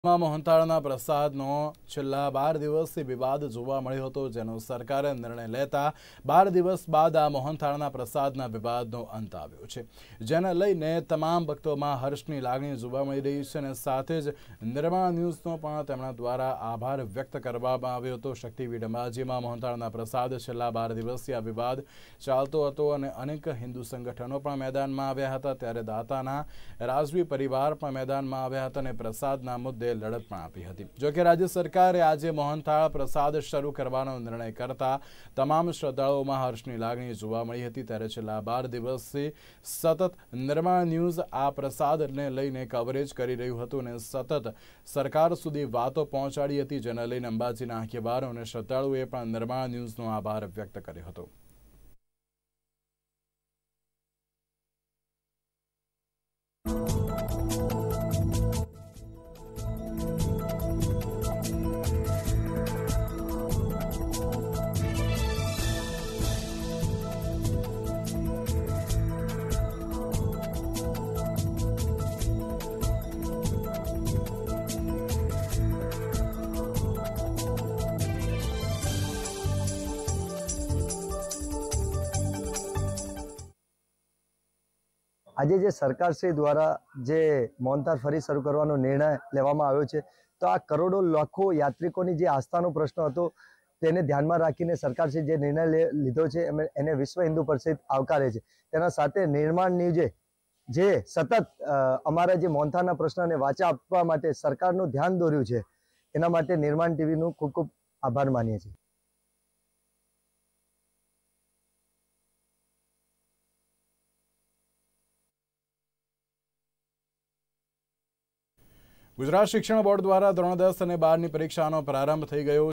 Ambaji : સતત બાર દિવસ સુધી અંબાજી પ્રસાદનું પ્રસારણ કરવા બદલ માઈ ભક્તોએ માન્યો નિર્માણ આભાર
Ambaji : સતત બાર દિવસ સુધી અંબાજી પ્રસાદનું પ્રસારણ કરવા બદલ માઈ ભક્તોએ માન્યો નિર્માણ આભાર । Nirmananews